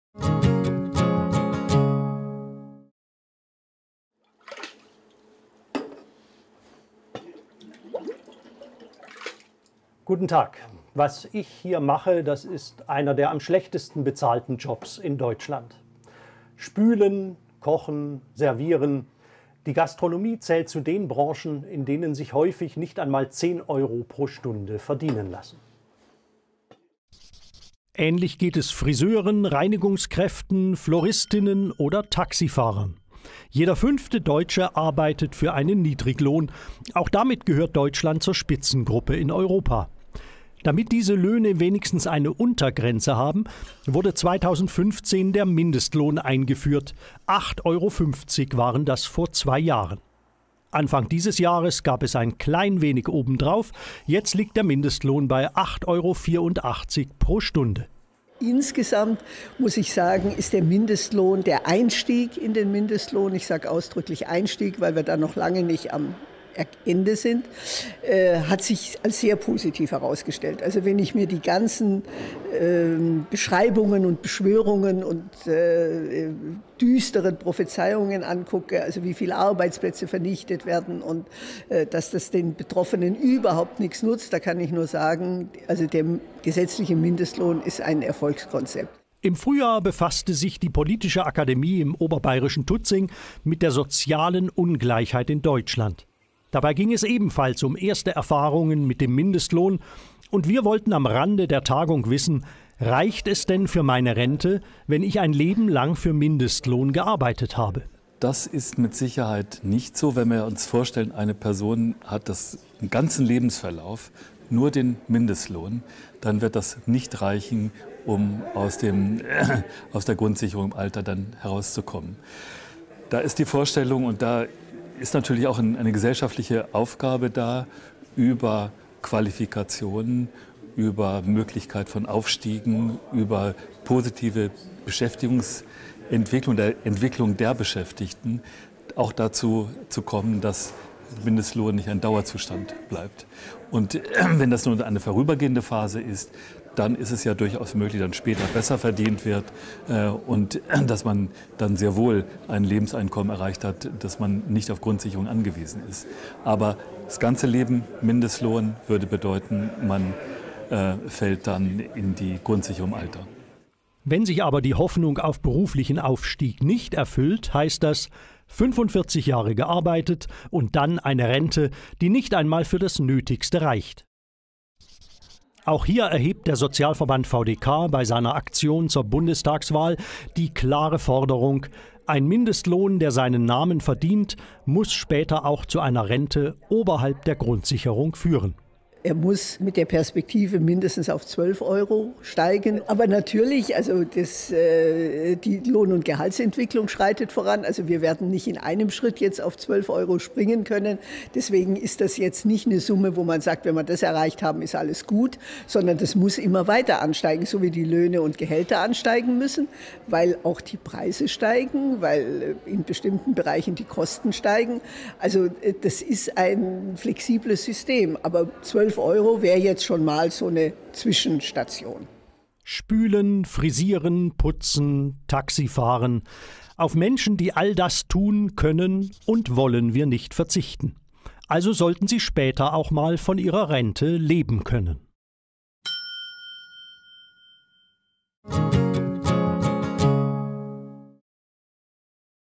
VdK-TV: Der Mindestlohn - zu wenig fürs Alter
Die Politik soll den Mindestlohn auf 12 Euro anheben - das fordert der Sozialverband VdK. Denn es zeigt sich, dass der jetzige Mindestlohn von knapp neun Euro nicht zum Leben reicht. Gerade Arbeitnehmer, die im Niedriglohnbereich arbeiten, brauchen einen Mindestlohn, von dem sie gut leben können. Arbeitnehmer brauchen aber auch deswegen einen hohen Mindestlohn, um als Rentner nicht von Alterarmut betroffen zu sein und von Grundsicherung leben zu müssen.

Mehr Informationen über den Sozialverband VdK und seine Positionen zu sozialer Gerechtigkeit finden Sie auf unsrer